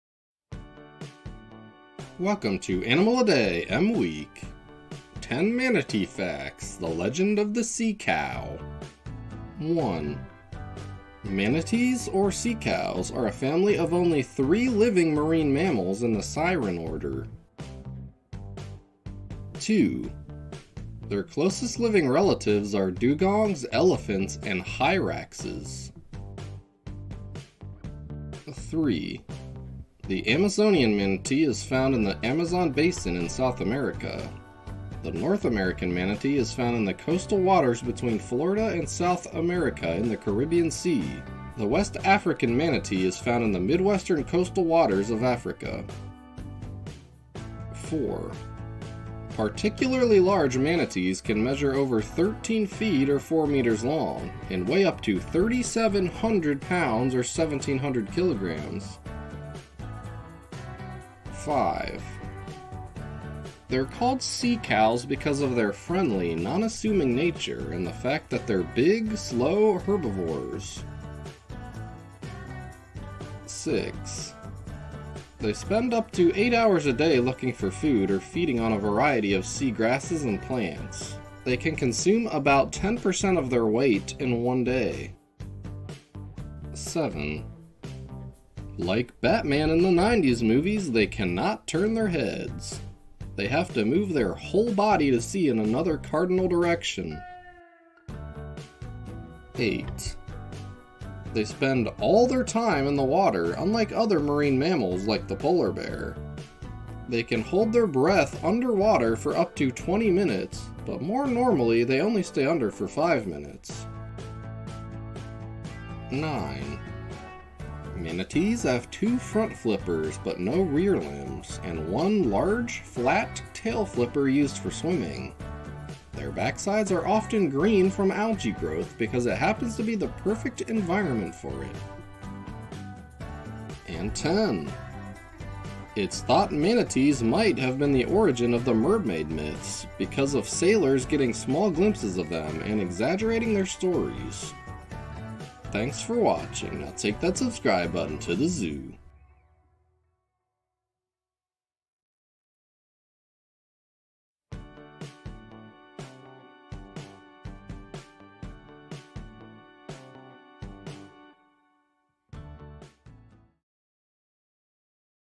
10 Manatee Facts - The Legend of a Sea Cow - Animal a Day M Week
Welcome to Animal a Day's M Week. Every day this week will have an animal that starts with the letter M. Today we learn about the tranquil heifer mistaken for mermaids, the Manatee.